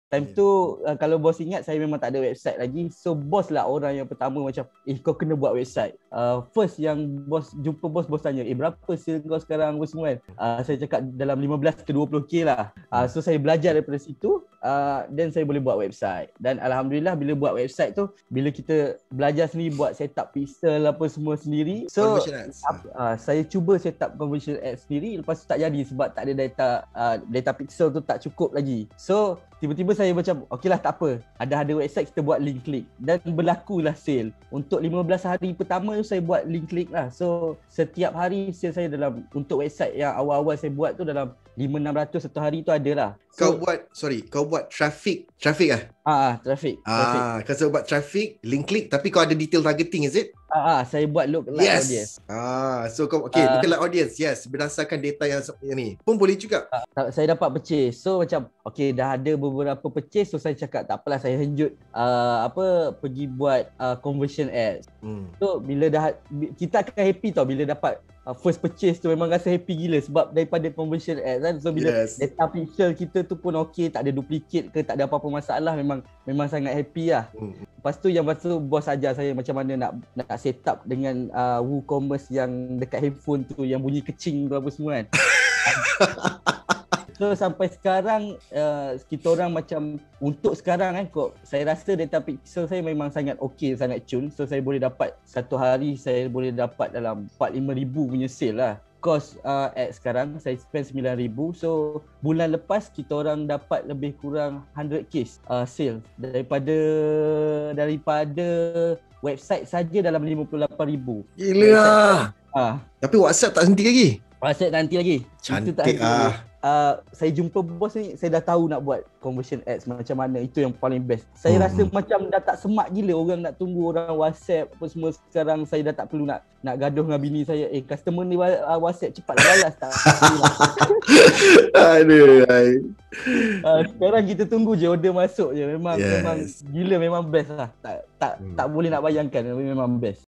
Testimoni Haas Akbar. Alumni Bootcamp Ecommerce Conversions dan Co-Founder Diane Lashes.
"Saya rasa data pixel saya memang sangat ok, sangat cun. Satu hari boleh dapat RM4000-RM5000 punya sales la. Sekarang kita tunggu order masuk je. Memang gila. Memang best la."
Haas Akbar, co-founder Diane Lashes dari usahawan Whatsapp kini mendapat incremental sales dari ecommerce sehingga lebih RM100,000 sebulan.